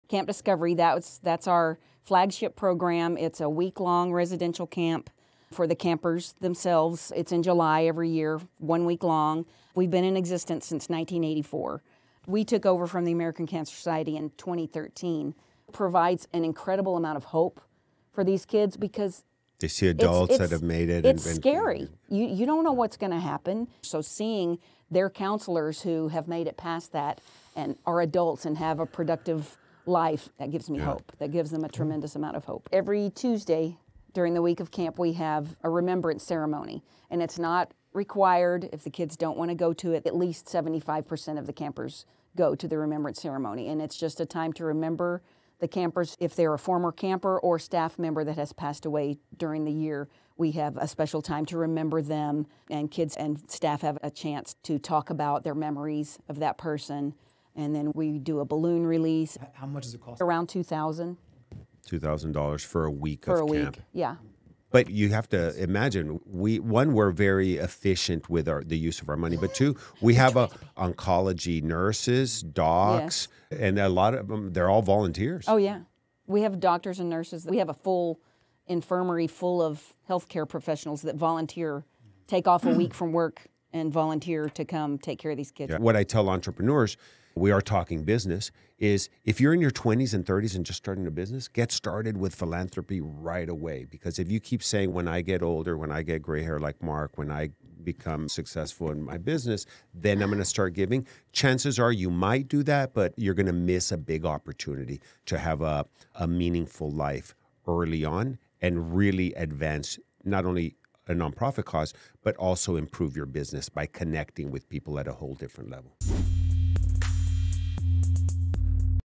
Event Ticket Sales Benefit Discovery Camps, a Local Non-Profit for Kids Battling Cancer
The net-proceeds from ticket sales to the "Let's Talk Business" Podcast mixer on May 16, 2024 will be donated to Discovery Camps, a non-profit organization that provides a unique camp experience for children ages 7-16 who are battling or have battled cancer.

Host:
✅ Marc Ebinger

Theme Song Credit:
Music from Uppbeat (free for Creators!):